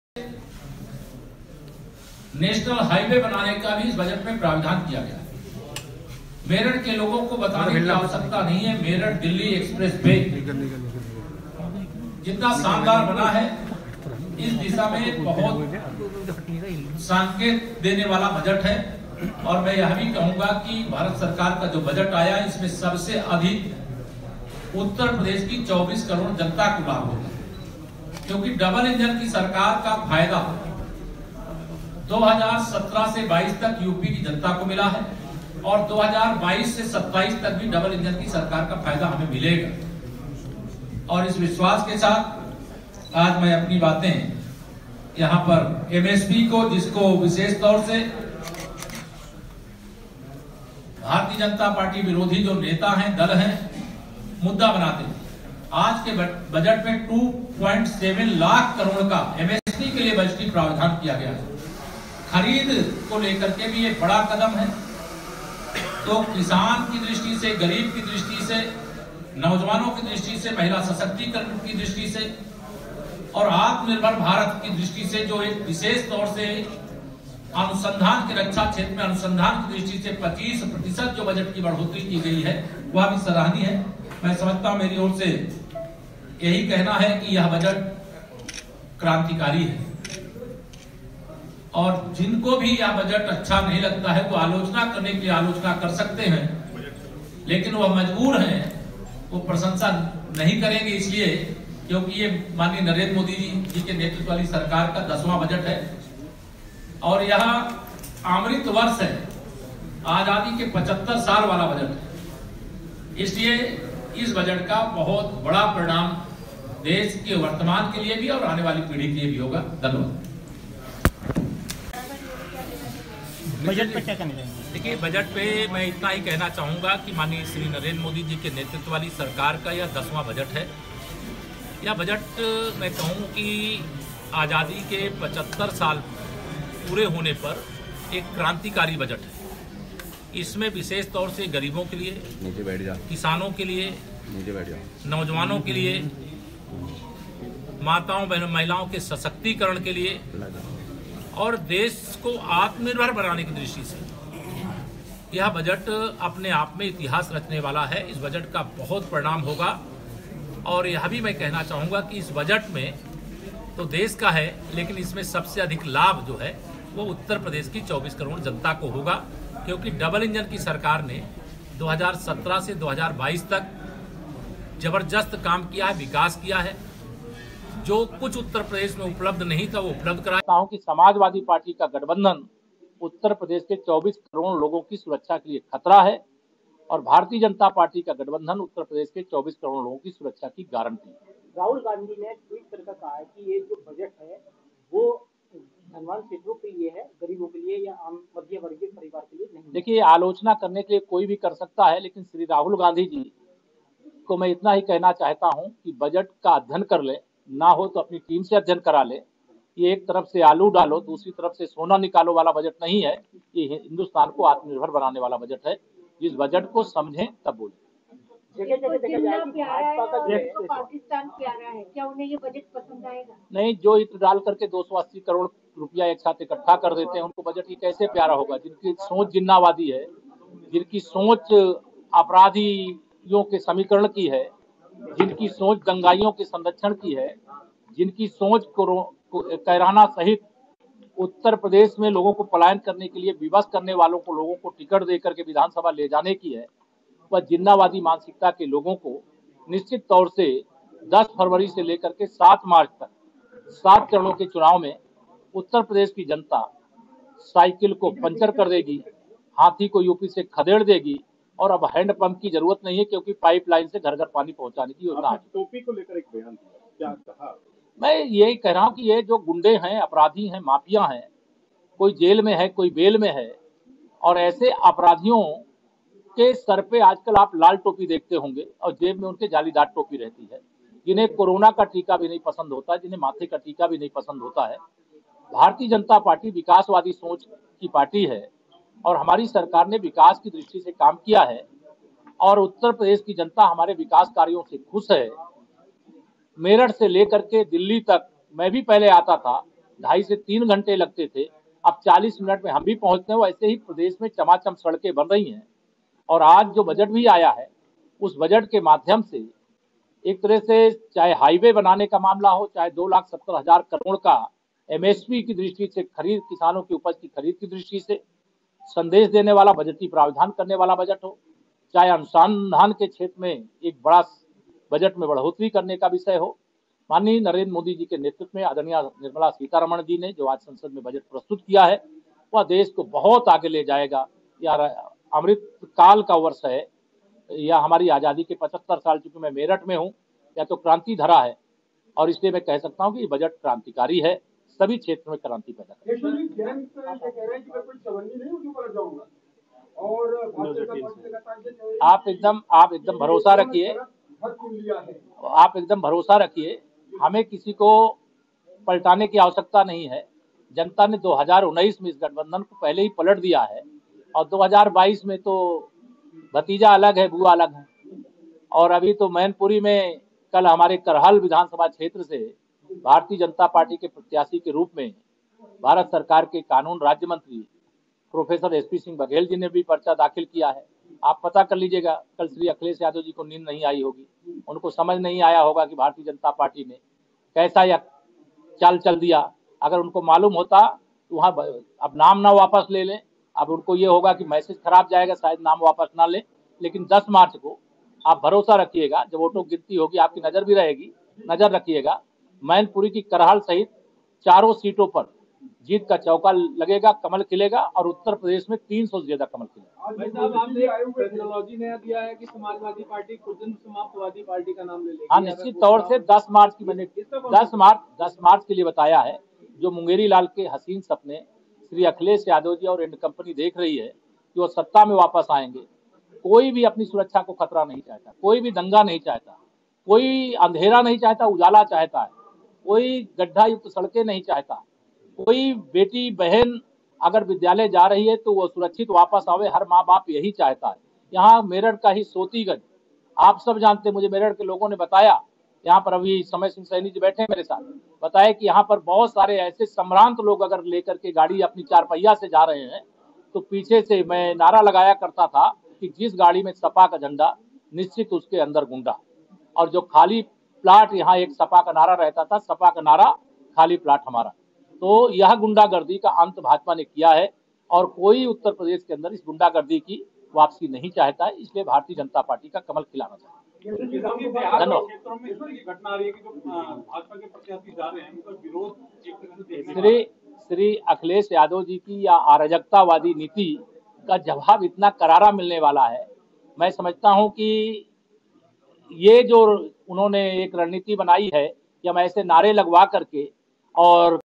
Akhilesh पर Keshav Prasad का हमला जेब में रखते हैं जालीदार टोपी ।। Indiapost­­­­ UP/UK
जेब में रखते हैं जालीदार टोपी, माथे का टीका भी नहीं पसंद: Akhilesh पर Keshav Prasad का हमला

Indiapost UP/UK